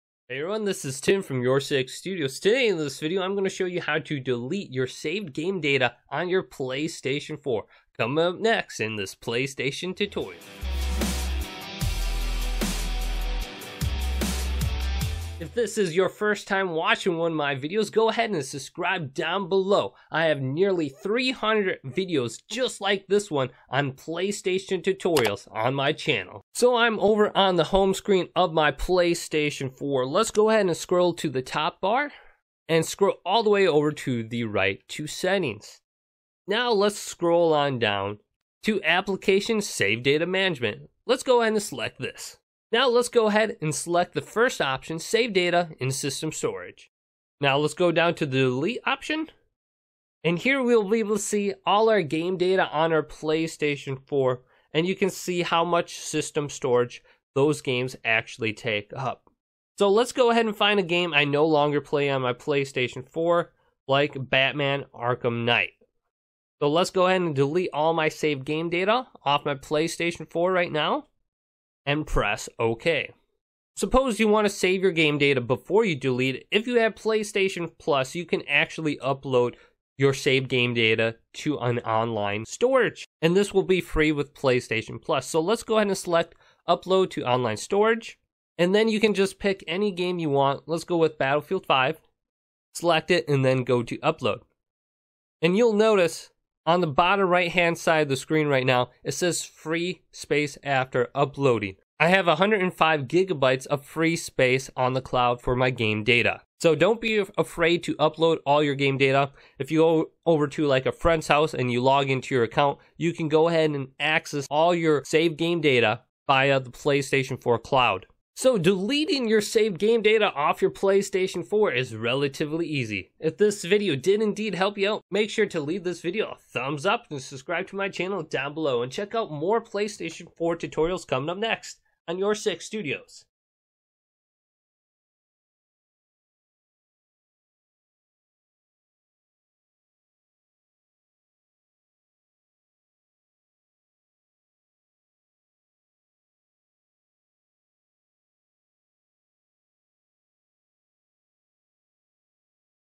How to Delete Saved Game Data on Your PS4 (Playstation Tutorial)
Do you want to know how to delete your saved game data on your PS4? In this playstation tutorial, I will show you how to delete your saved game data and also, how to save your game data to the PlayStation Cloud, so you can access your saved game data anywhere in the world. First go to settings. Now go to Application saved data management. Go to the first option, saved data and system storage. Under here, you will be able to delete your Saved Game data. ...